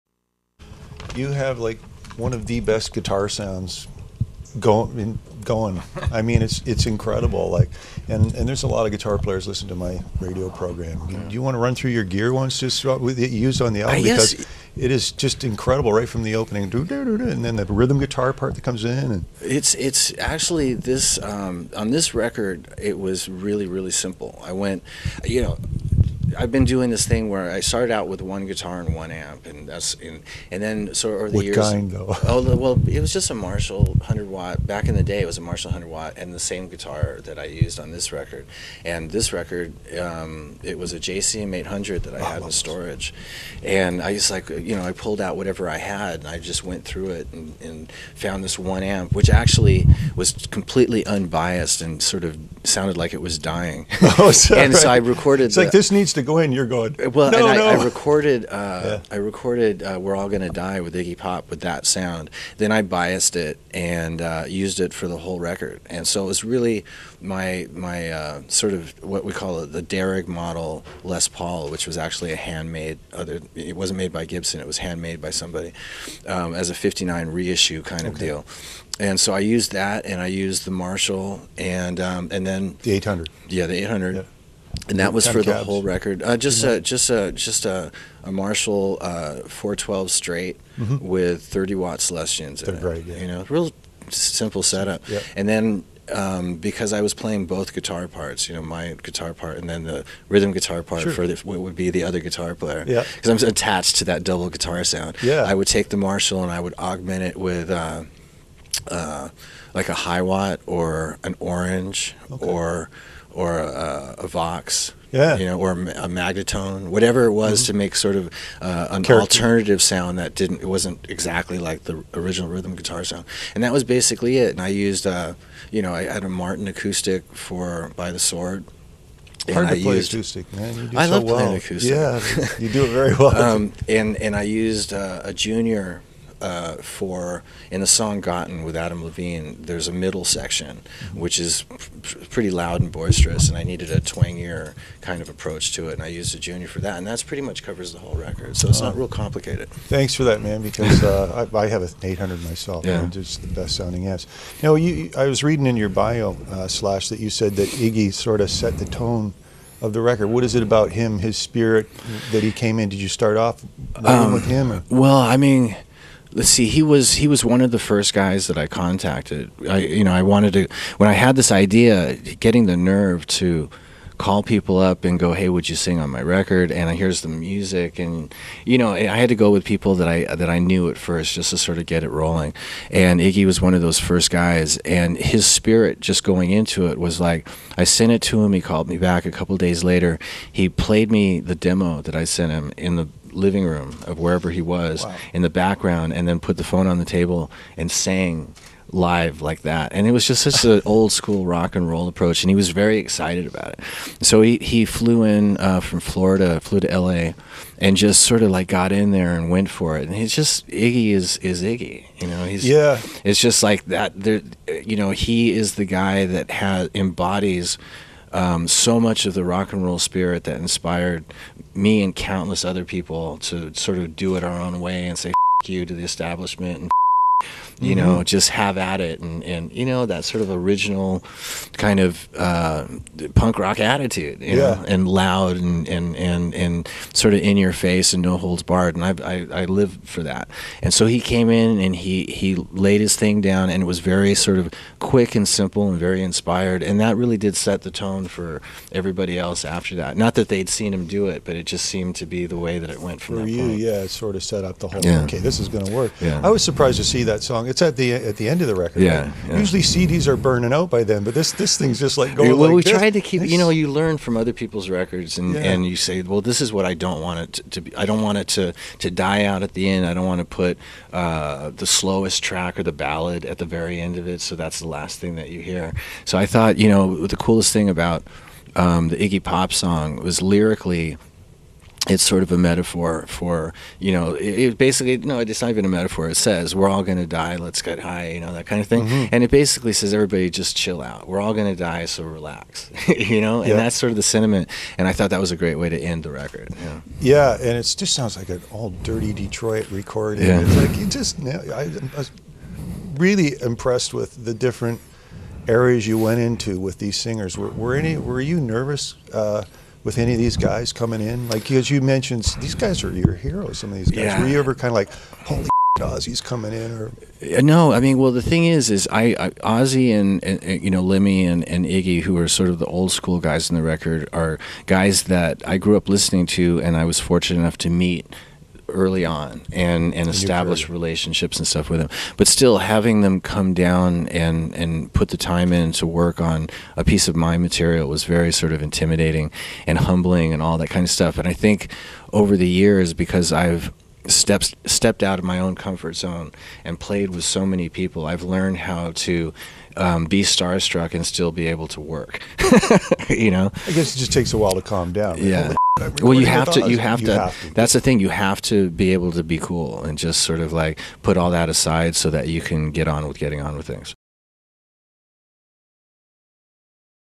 Kim Mitchell interviews Slash - part 1 of 3
Q107's Kim Mitchell sits down with legendary guitar player Slash. In this clip (part 1 of 3) Kim and Slash discuss what kind of guitar gear Slash uses on his new album and what it was like working with legends like Iggy Pop and Ozzy Osbourne.